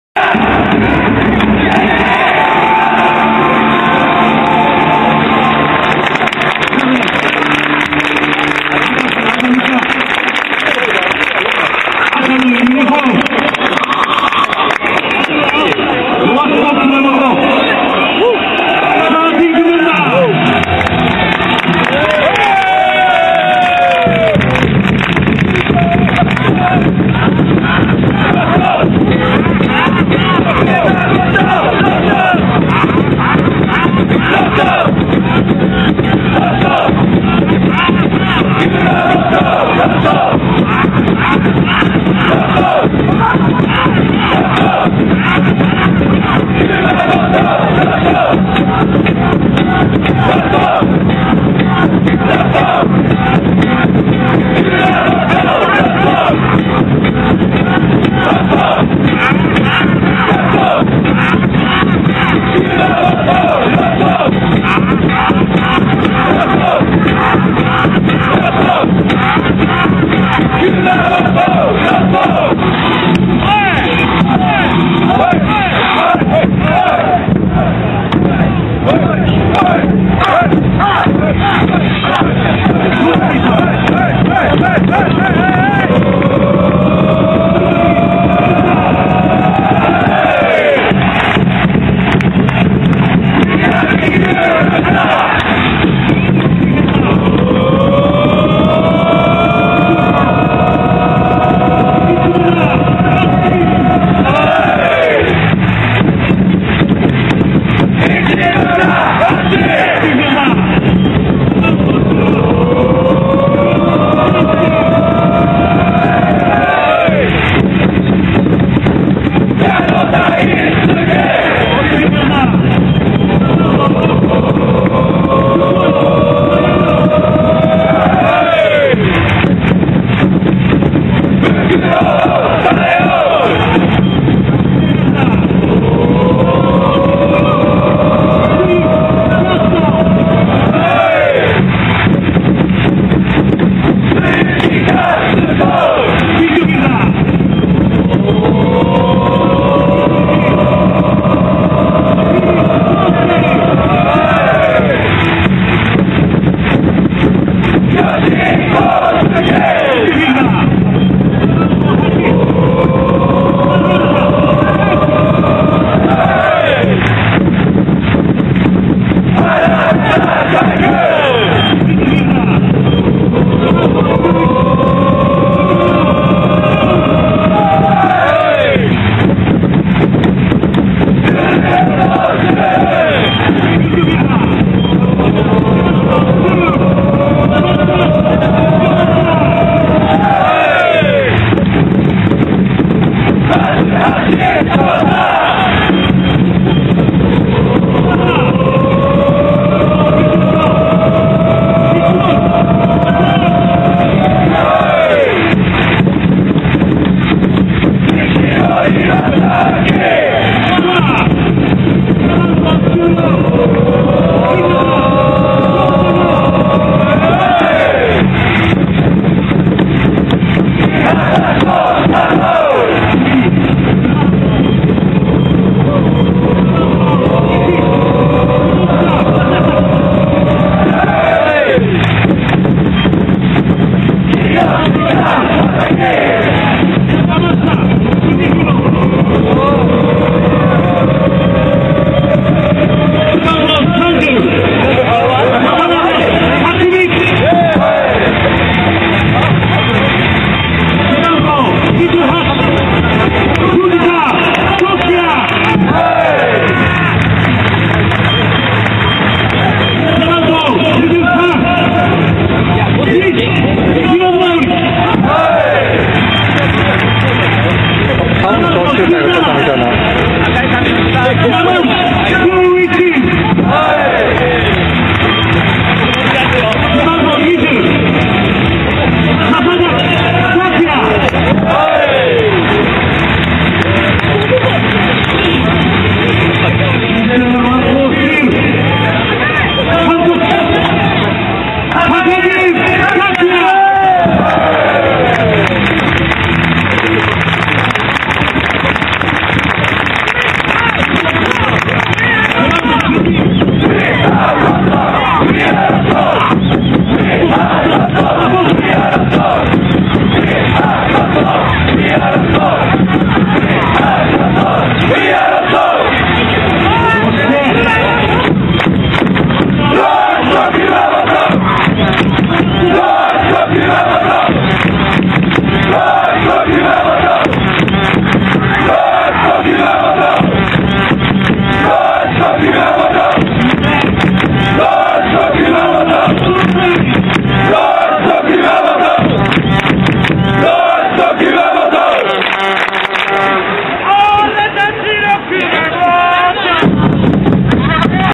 2010 ロアッソ熊本 選手紹介
第1節(対ジェフ千葉戦)。
去年の映像のマイナーチェンジっぽいです。
というより使いまわry
控え選手の1人当たりの紹介時間が若干減ったので
サポのほうもチト戸惑い気味でした。
DJコバは...もう少し頑張りましょう（笑)
まぁ初めてだし緊張もあったのかな？